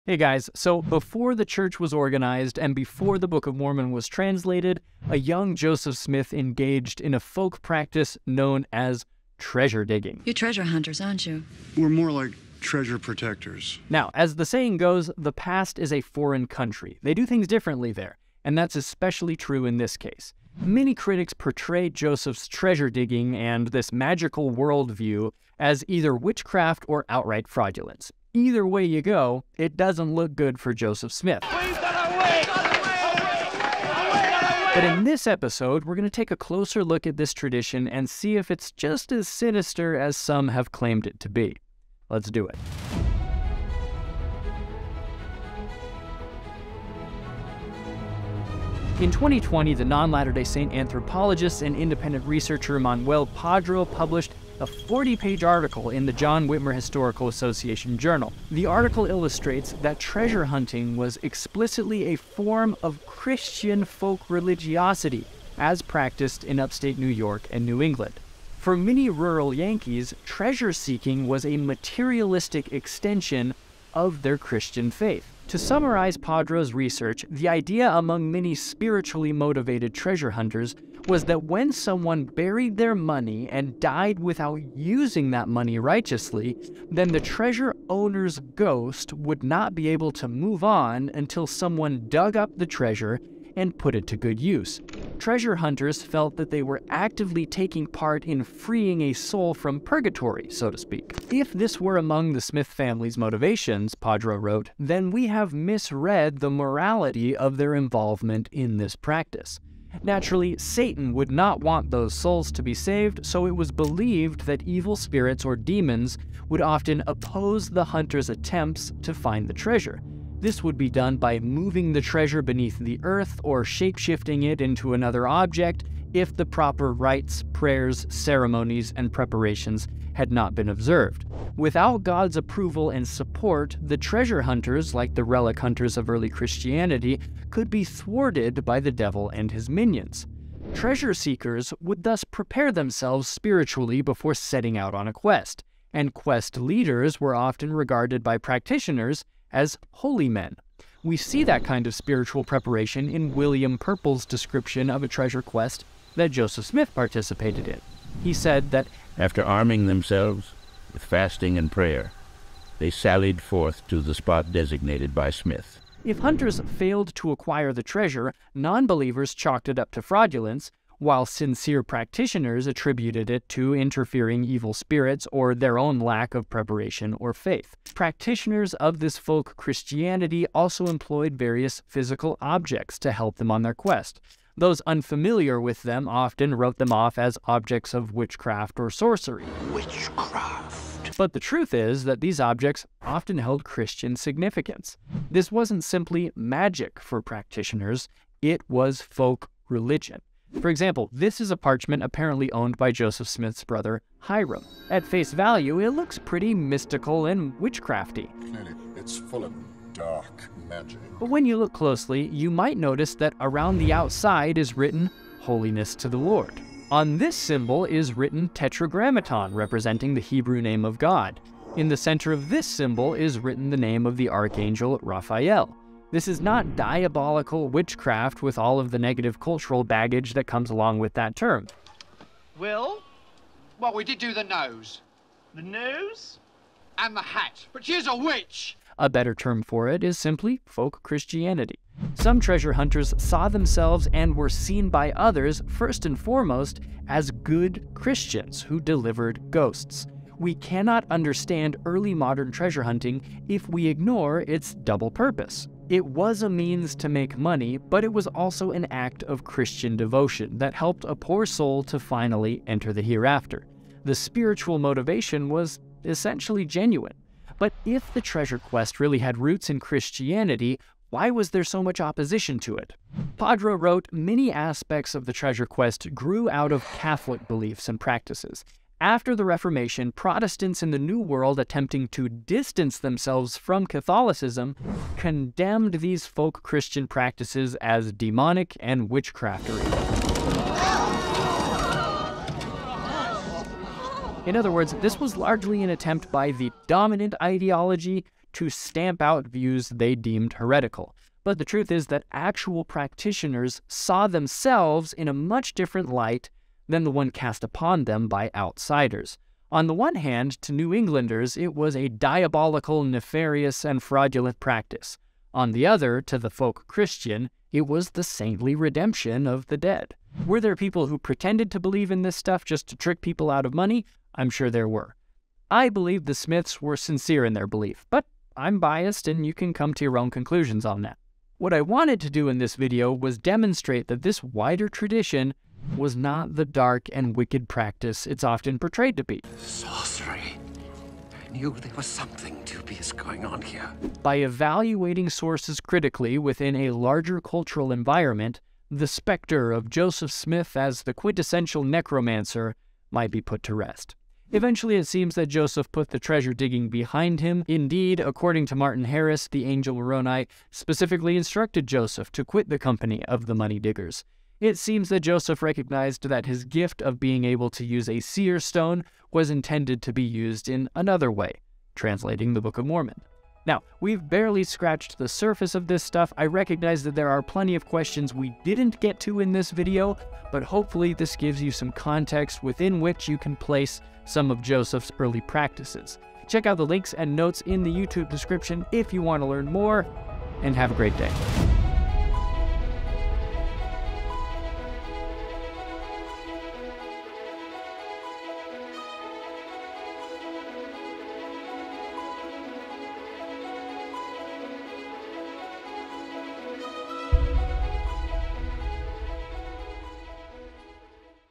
The truth about Joseph Smith, treasure-digging, witchcraft, magic, and the occult. Ep. 193
Was Joseph Smith a treasure-digger? And if so, what does that mean? Does that mean he practiced “witchcraft”? Or was he a necromancer? Or does this qualify as being involved in the occult? In this episode, David explores some new research that sheds some much-needed light on these “sinister” practices from the past. Spoiler alert: It’s not quite as sinister as some would have you believe.

— “Redemption: The Treasure Quest and the Wandering Soul,” by Manuel Padro in the John Whitmer Historical Association Journal (Fall/Winter 2020, Vol. 40, No. 2)
— “Cunning and Disorderly: Early Nineteenth-Century Witch Trials of Joseph Smith,” by Manuel Padro in Dialogue Journal (Winter 2021, Vol. 54, No. 4)
— “Cunning Distortions: Folk Christianity and Witchcraft Allegations in Early Mormon History,” by Manuel Padro in Journal of Mormon History, Vol. 49, No. 1, 2023
— “Joseph Smith and Money Digging,” a short read by Richard Bushman (BYU Studies)
— “Treasures, Witches, and Ancient Inhabitants (D&C 111),” by Craig Ostler (BYU Studies)
— “Martin Harris’ 1859 Interview with Joel Tiffany on Early Events in Mormonism,” via Doctrine and Covenants Central
— “Joseph Smith's money digging activities and how it relates to his character,” via FAIR
— Appendix 1: Agreement of Josiah Stowell and Others, 1 November 1825,” via The Joseph Smith Papers

Notes:

— "Some symbols on Hyrum Smith’s parchment match symbols from Reginald Scot’s 1584 book, 'The Discoverie of Witchcraft.' Thus, some claim that the Smiths clearly understood and recognized that they were engaged in 'witchcraft' (after all, it’s in the title of the book, right?). Rest assured that this is not a safe assumption to make, for reasons that I am not currently at liberty to discuss here. For more info on this question, you'll want to keep tabs on Manuel Padro's upcoming publications."

— We really don’t even know just how deeply Joseph was involved in this stuff. Joseph’s biographer, Richard Bushman, wrote,

“At present, a question remains about how involved Joseph Smith was in folk magic. Was he enthusiastically pursuing treasure seeking as a business in the 1820s, or was he a somewhat reluctant participant, egged on by his father? Was his worldview fundamentally shaped by folk traditions? I think there is substantial evidence of his reluctance, and, in my opinion, the evidence for extensive involvement is tenuous. But this is a matter of degree. No one denies that magic was there, especially in the mid-1820s." Source

You’ll notice that even here, Bushman chooses such terms as “folk magic” to describe Joseph’s practices. I wonder if he might have chosen different words (like “folk-Christianity”) had he been aware of this more recent research coming from Manuel Padro. The evidence seems to suggest that practitioners didn’t view their practices as magical, but miraculous/folk-religious.

— Joseph seemed to be pretty open about his money-digging past. In one interview he was asked, “Was not Jo Smith a money digger[?]” to which he replied, “Yes, but it was never a very profitable job to him, as he only got fourteen dollars a month for it.” Source

— I also liked this quote from Padro’s article (pg. 77): “When compared to Pennsylvania Dutch accounts, it is obvious that these New Englander examples demonstrate a misrepresentation of the actual practice of treasure hunting by labeling it as both witchcraft and fraud. This begs an important question: If some of the best literary minds of New England cannot accurately describe the treasure quest of Teutonic-Americans, are the descriptions of the treasure quest recorded by average New England Americans trustworthy either? The answer is no. These sources were not ethnographers describing what they had seen, observed or participated in, but rather bigots describing what they imagined other people did, utilizing their own cultural views of witchcraft and fraud as the building materials for an imaginary event that they projected onto a group they saw as foreign.”